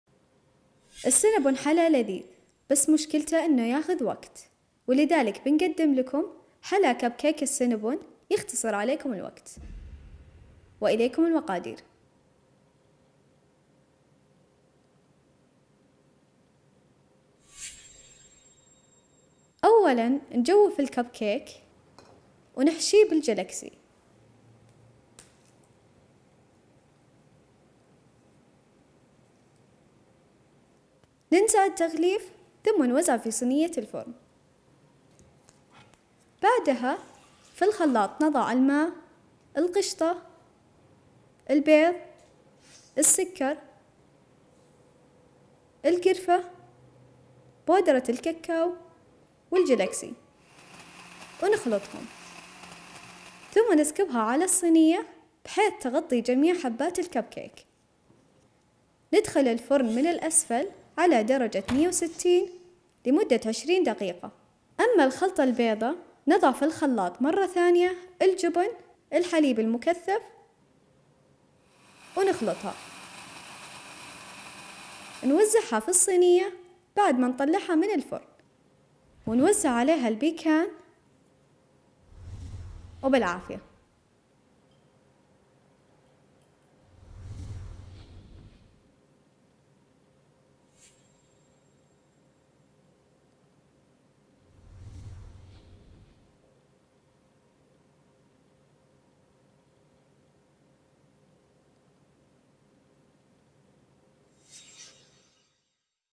وجبات_15_ثانية | كب كيك السينامون 15s | Cinnabon cupcakes #
وجبات 15 ثانية، المنصة الأولى لتعليم الطهي عبر وصفات فيديو قصيرة!

اسم المسخدم في سناب شات: snap15s

كب كيك السينامون - Cinnamon Cupcakes

المقادير:
١٥ كب كيك سادة أو شكولاته
٣ ألواح شكولاته
بيكان أو جوز أمريكي

التغطية ( في الخلاط ):
٤/١ كوب ماء
١ قشطة
بيضة
٤/١ كوب سكر
١ ملعقة كبيرة قرفة
١ ملعقة كبيرة بودرة كاكاو
١ لوح شكولاته

الطبقة البيضاء ( في الخلاط ) :
٢/١ علبة حليب مكثف (٣٩٠غ)
٤ حبات جبن كيري

الطريقة:
نجوف الكب كيك ونحشية بقطعة من الشكولاته بعد نزع الغلاف ثم نصفه في صينية الفرن.
نخلط مقادير التغطية ونوزعه على جميع حبات الكب كيك.
ندخلها الفرن على درجة ١٦٠ من الأسفل لمدة ١٥ -٢٠ دقيقة.
نخلط مقادير الطبقة البيضاء ونوزعها على الكب كيك بعد أن تخرج من الفرن.
وأخيرًا نوزع عليها البيكان أو الجوز الأمريكي.

Ingredients:

15 Cupcakes ( Vanilla OR Chocolate )

3 Chocolate Bars

Pecan nuts

Topping ( In Blender ) :

¼ Cup Water

1 Table Cream

1 Egg

¼ Cup Sugar

1 tbsp. Cinnamon

1 tbsp. Cocoa Powder

1 Chocolate Bar

White Topping ( In Blender ) :

½ Can of Sweetened Condensed Milk

4 Kiri Cheese

Preparation:

make a hole in the middle of your cupcakes with either a small melon baller or special cupcake hole puncher.

Then fill in each hole of your cupcake with a piece of chocolate.

Remove cupcakes papers, and put it in baking pan .

Mix all topping ingredients, then drizzle on cup cakes .

Put in oven on 160ºF for 15 - 20 minutes.

When cooked & cool down , mix white topping ingredients, and drizzle on cupcakes .

Finally, Sprinkle the pecans evenly over the cupcakes.